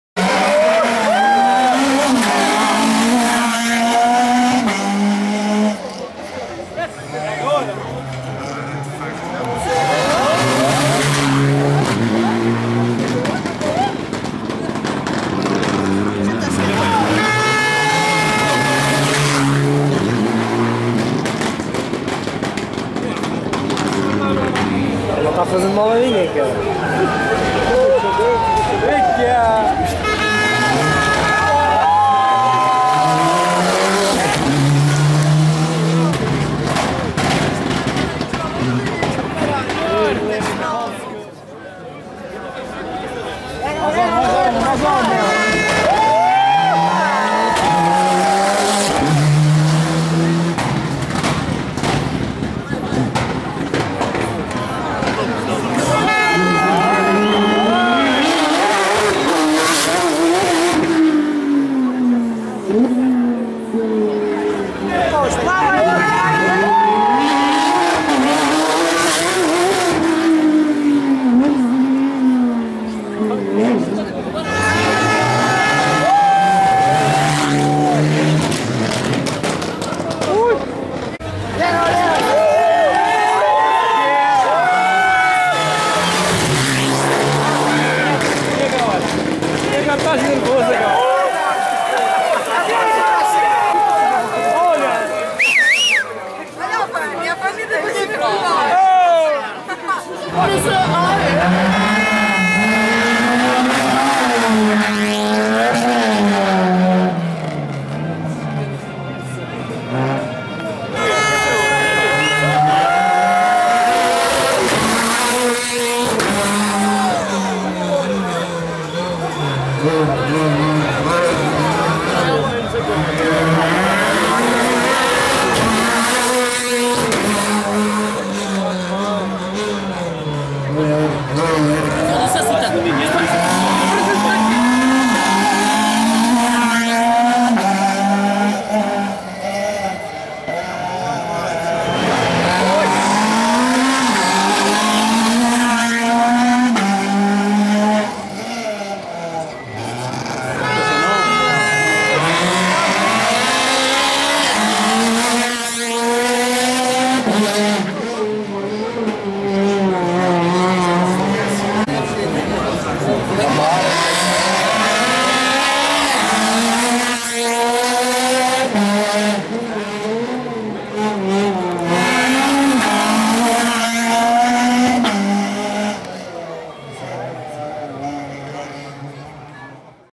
Prova Especial nocturna em Ponta Delegada - Rali São Vicente 2014
Algumas passagens da prova nocturna.